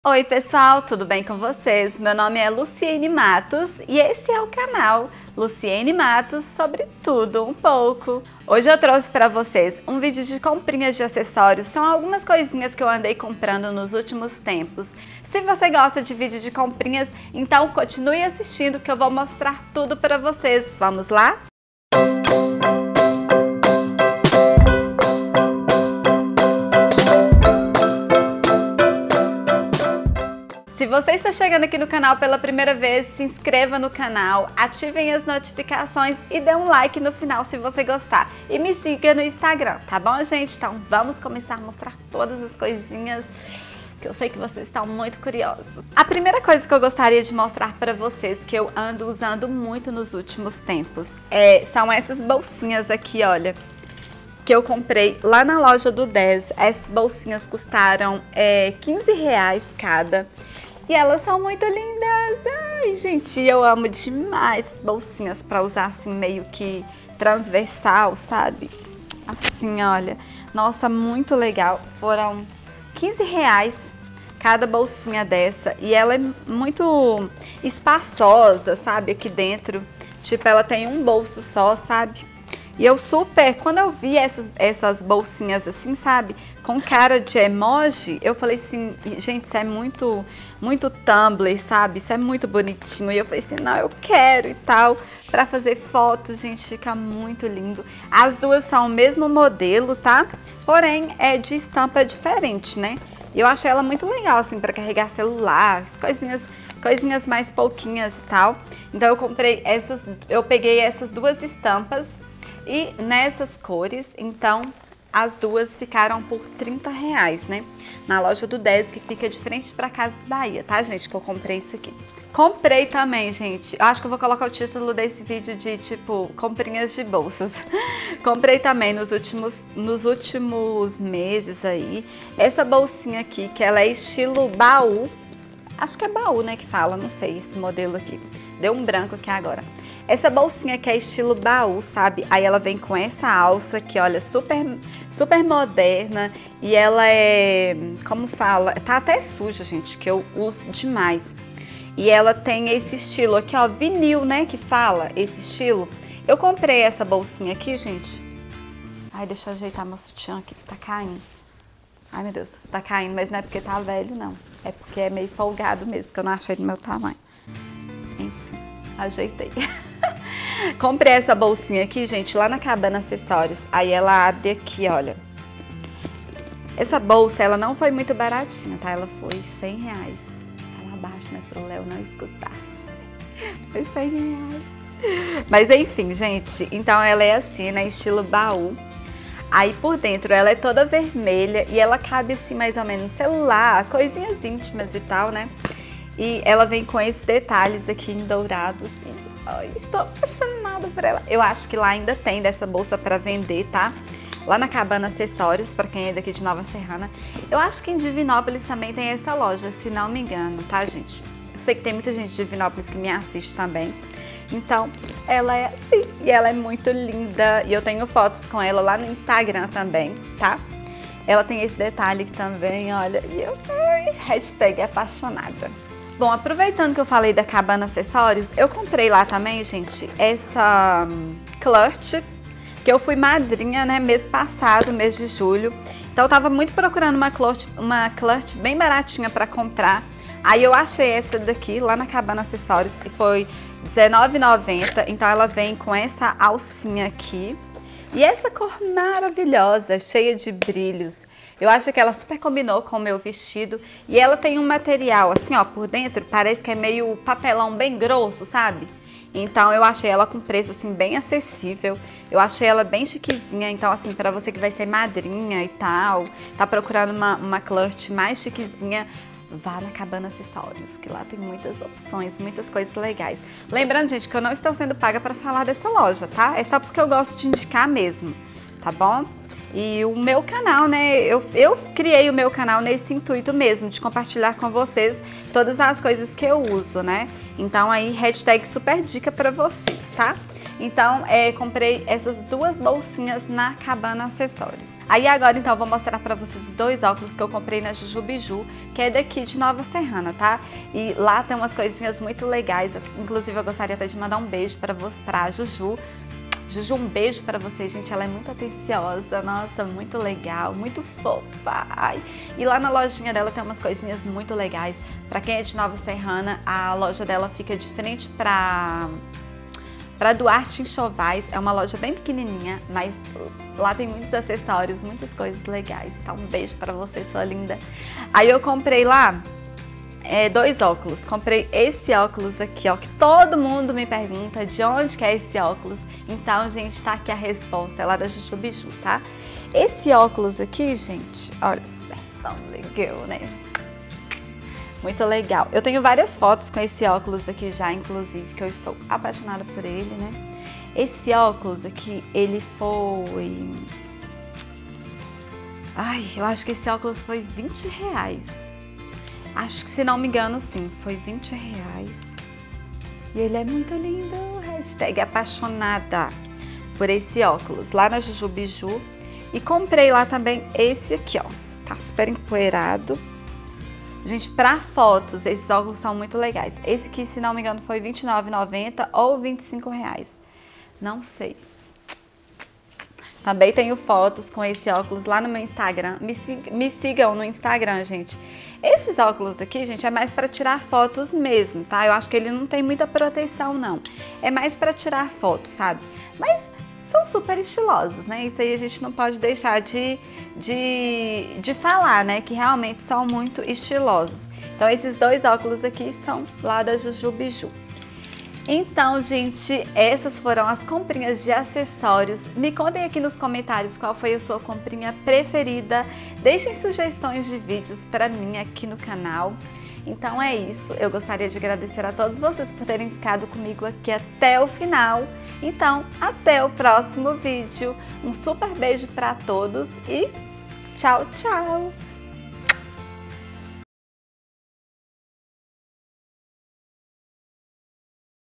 COMPRINHAS DE ACESSORIOS E BOLSAS
OIE!!!VÍDEO NOVO NO AR PARA VCS!!!TODAS AS PEÇAS SÃO AQUI DA MINHA CIDADE.ESPERO PODER INSPIRAR VOCÊS.
OBS:VÍDEO GRAVADO NO INÍCIO DE AGOSTO.SÓ AGORA TIVE TEMPO DE PUBLICAR DEVIDO A FALTA DE TEMPO.
Duvidas e sugestões podem deixar nos comentários...
Fanpage: Luciene Matos sobre tudo um pouco
Instagram: Luciene Matos sobre tudo um pouco